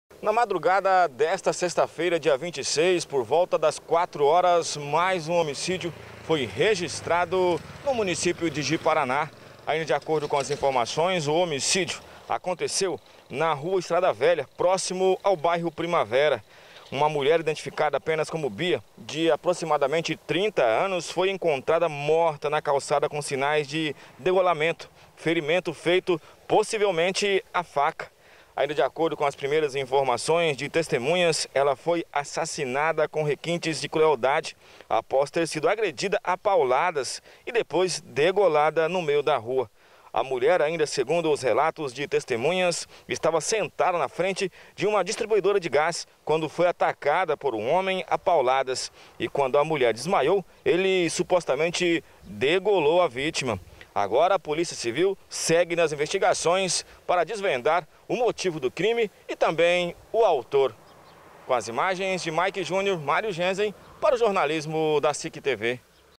Matéria Mulher é degolada no bairro Primavera em Ji Paraná
Créditos:
Reportagem: Mario Jensen
Cinegrafista: José Henrique
Edição: Mayke Júnior